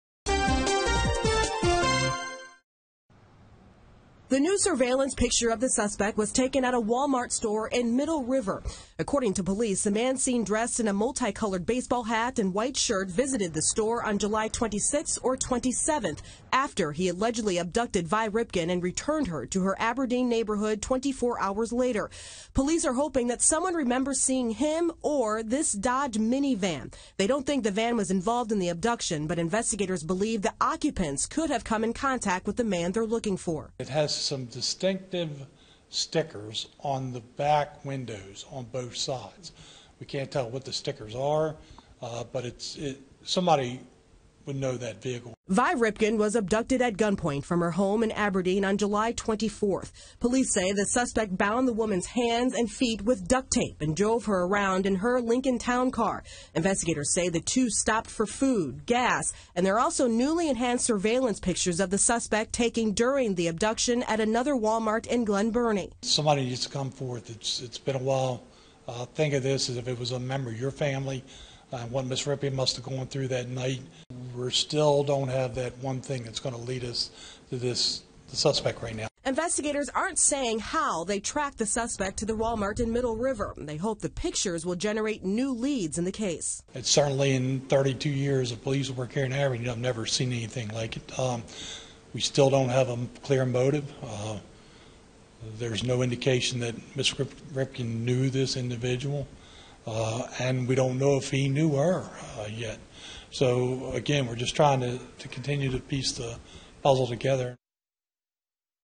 New person of interest photos released in Vi Ripken abduction
Police in Harford County continue to seek clues into the armed abduction of the mother of Baltimore Orioles Hall of Famer Cal Ripken Jr. over the summer. Kai Reed has an update on the newest images police are releasing of a potential suspect in the